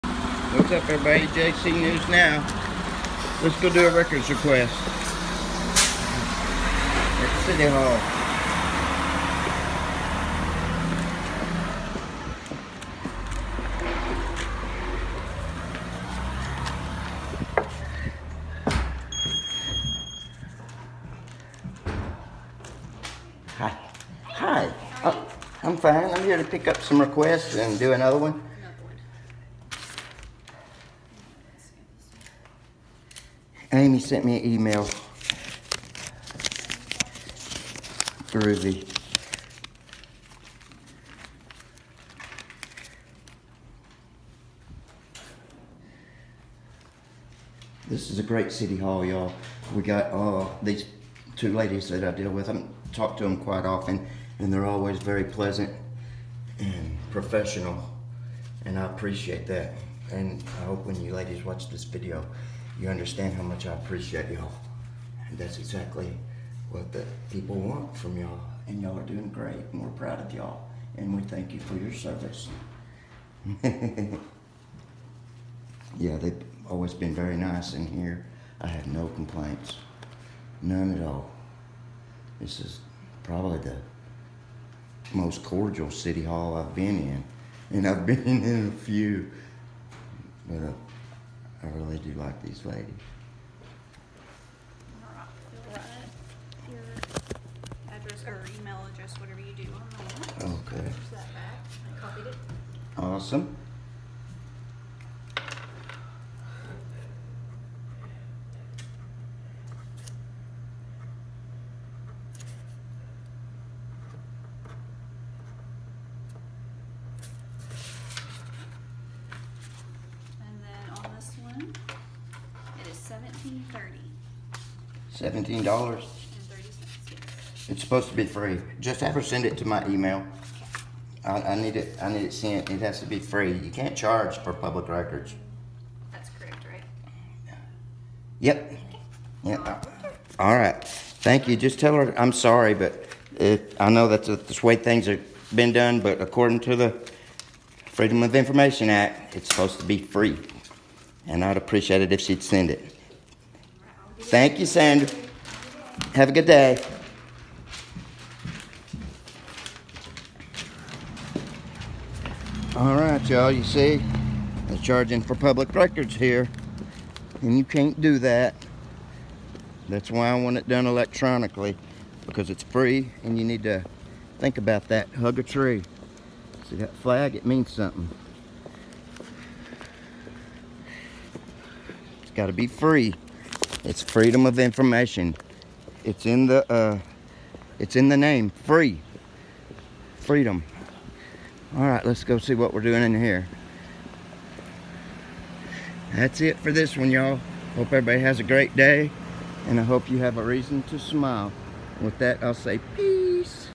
Trying To Charge Me For Public Records
I have to say that free is in the name of the act. Freedom of Information Act allows you to get public records.   They are charging me for paperwork.  I specifically asked them to email the information to me as to avoid physical documents.  Hug a tree.